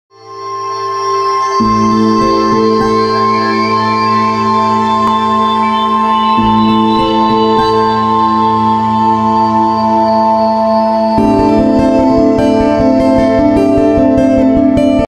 Expect more from your floor.
Silikal industrial flooring & commercial flooring logo intro.
Flooring you can count on that lasts and lasts